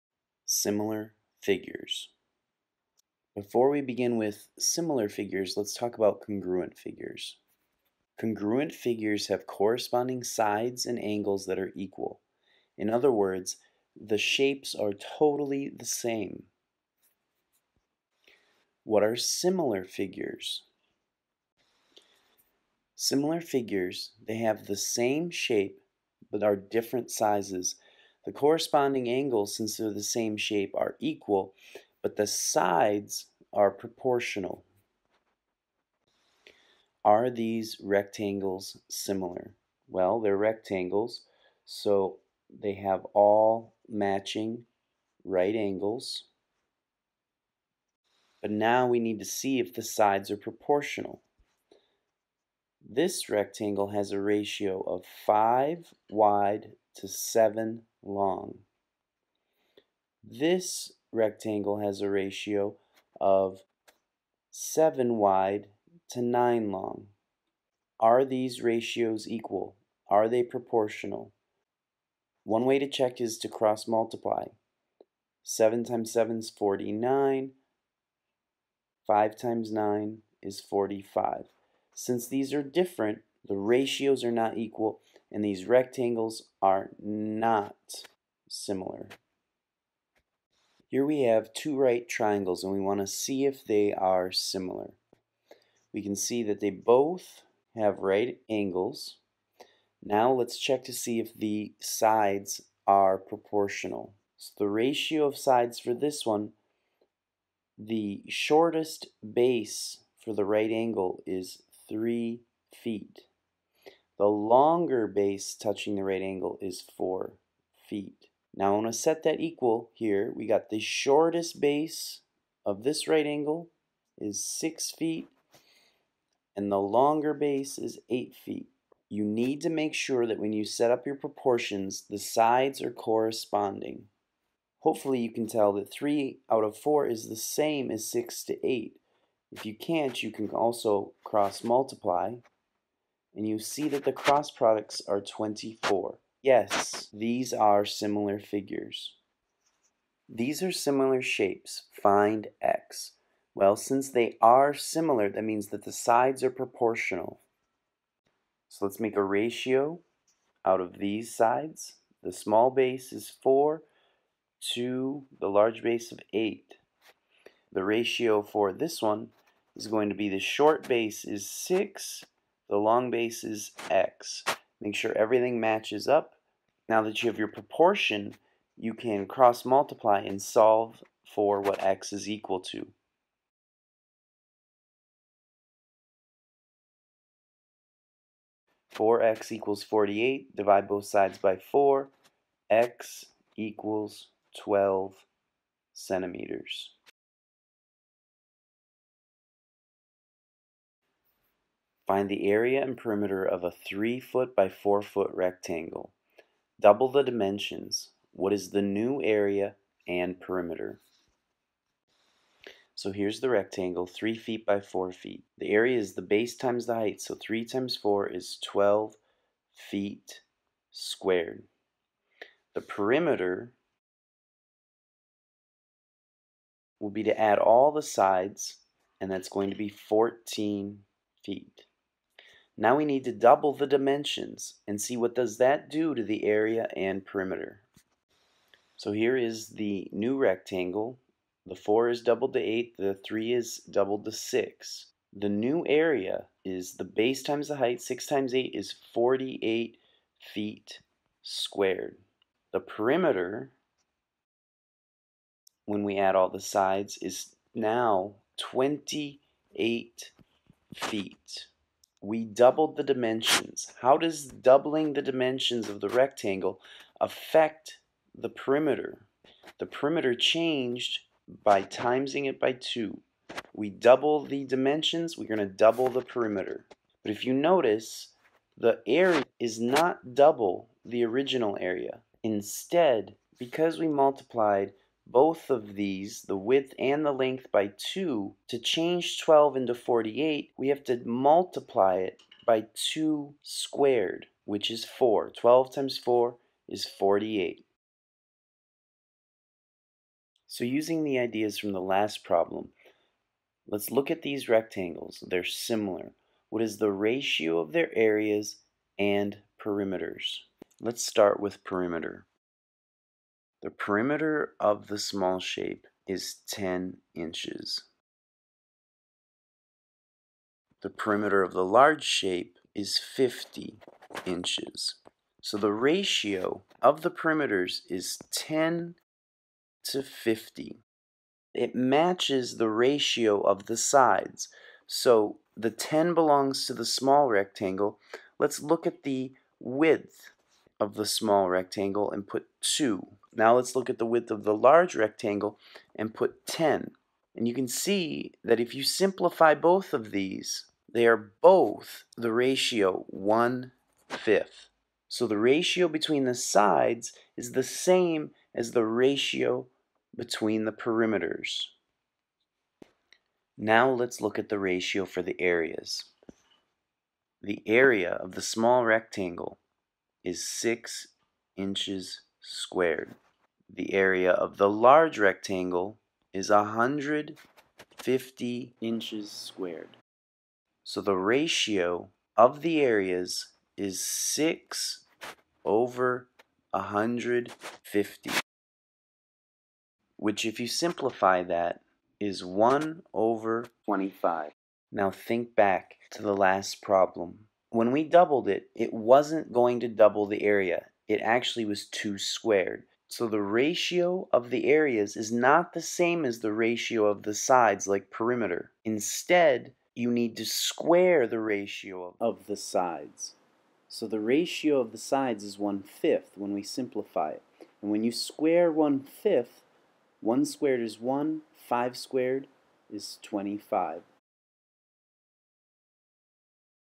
Similar Figures
Similar Figures.  Also covers how the perimeter changes when you change the dimensions...